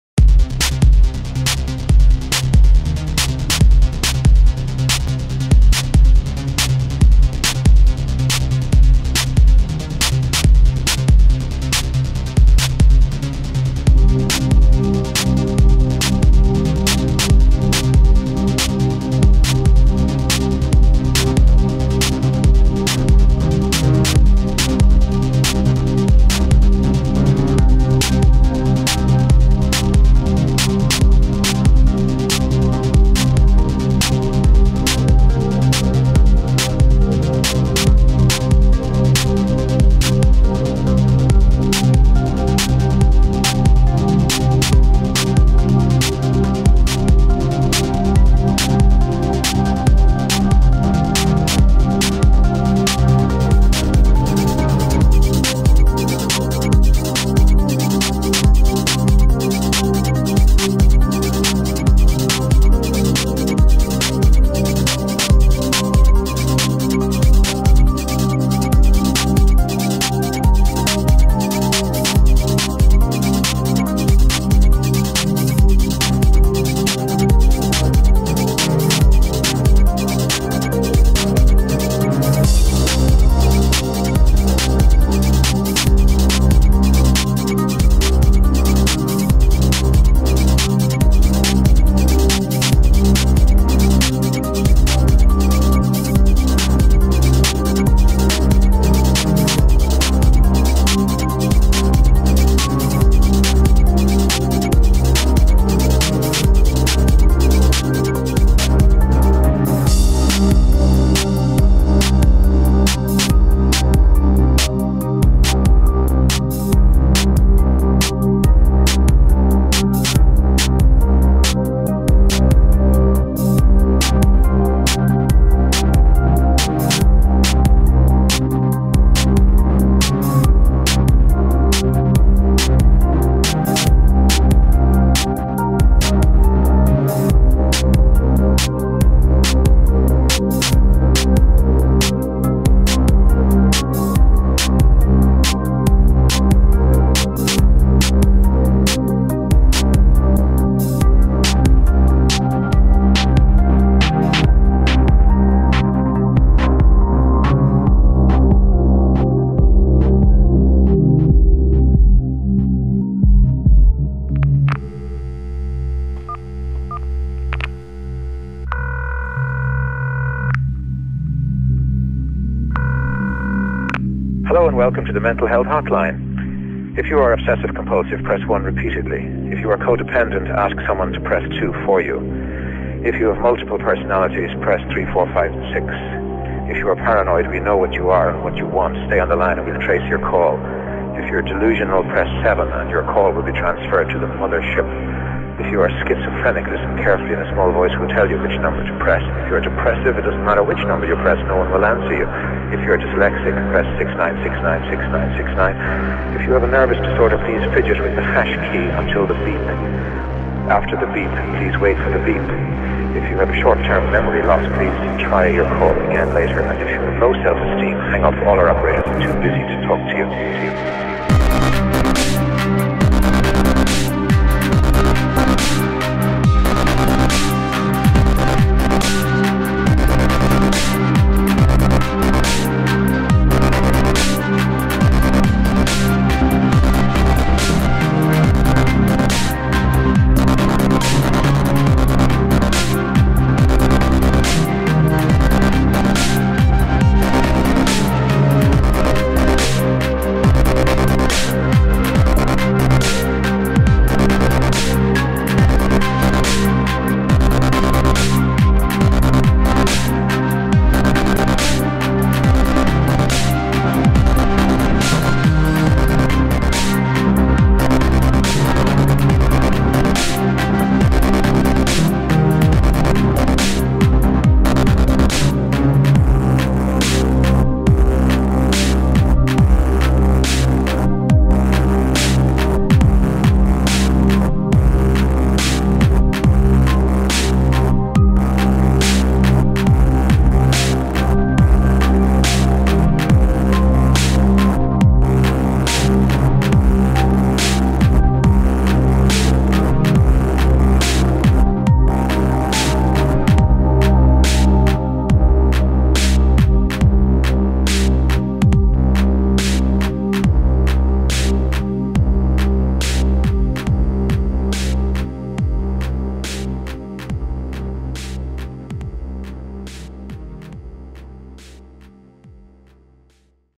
Art school demo reel (2013)
just a compilation of my art school projects for a convention we went to.